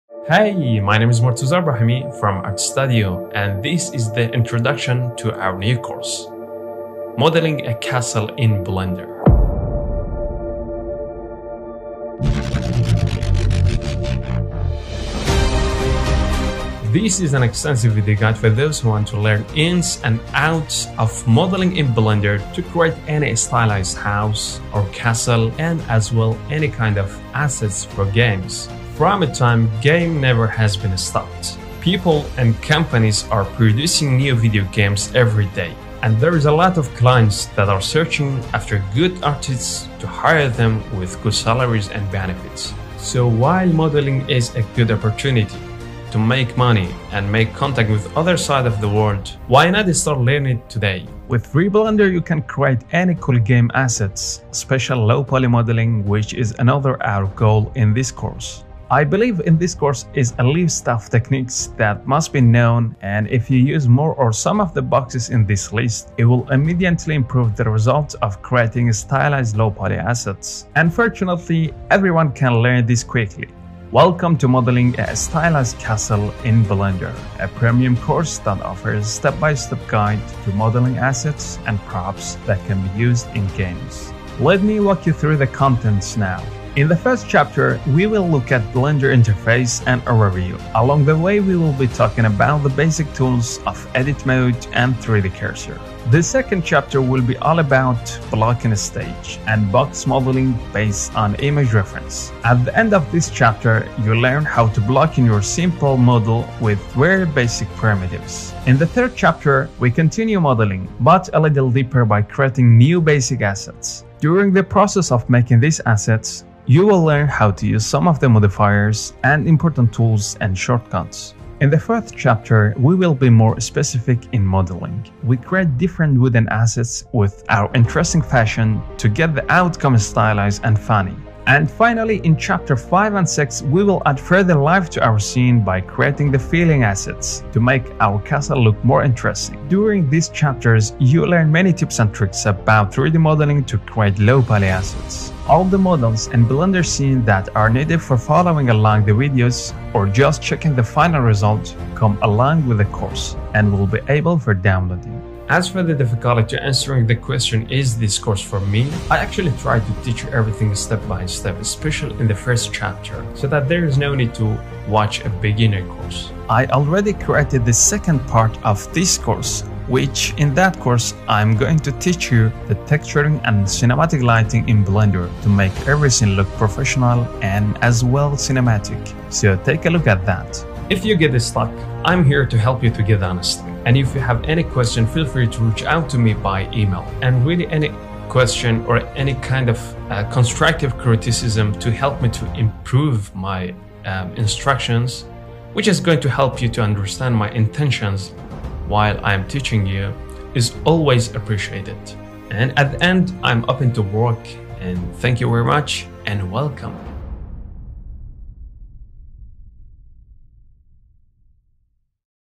Introduction To Modeling a Castle in Blender / low-poly game assets
This is an extensive Modeling guide in blender to make stylized scene for games

you can rich out to me by email in case If you have a proposal for cooperation idea: